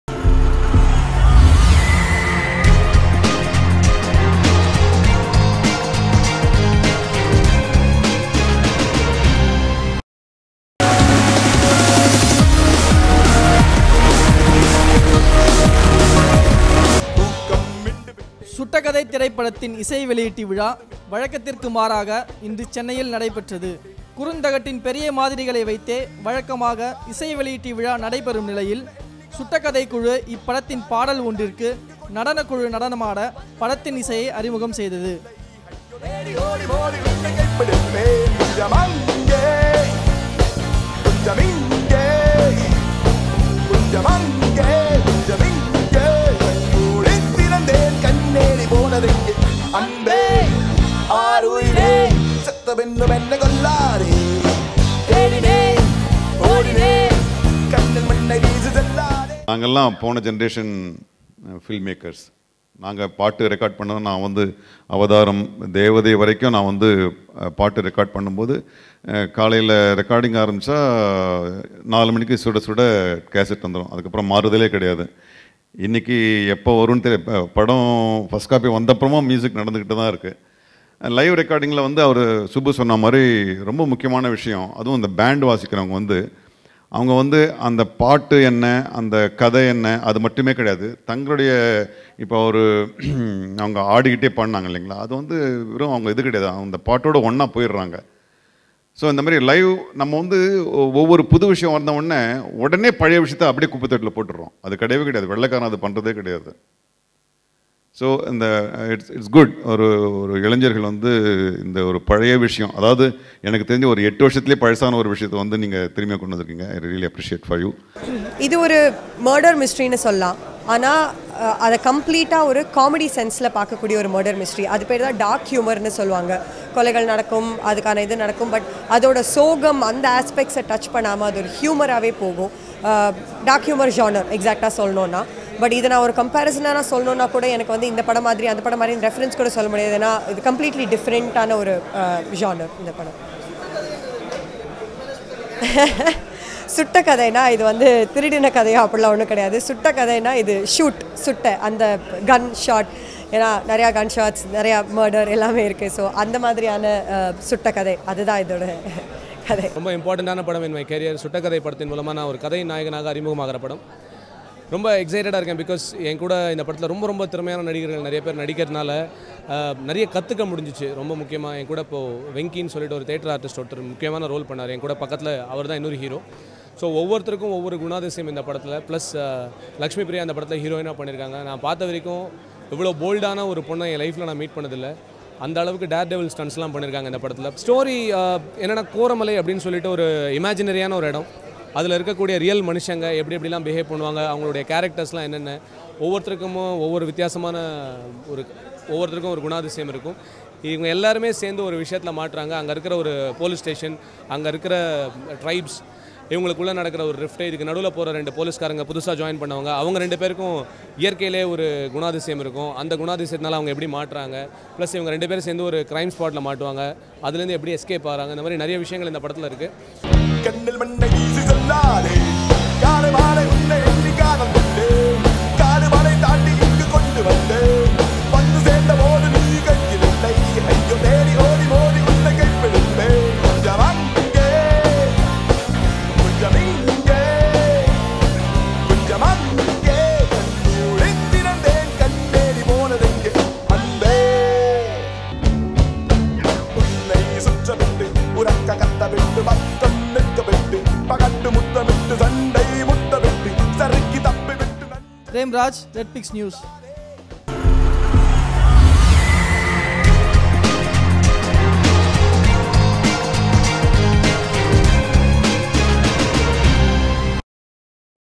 Sutta Kadhai yet another black comedy following the success of Soodhu Kavvuum [RED PIX]
Sutta Kadhai is an upcoming Tamil dark comedy film directed by Subu, a debuted director . Nassar, Balaji, Venkey, Lakshmi priya and M.S Baskar are play a lead role .The plot of the story is, two police constable try to solve a murder mystery in a village. This comedy movie is schedule to release in July 2013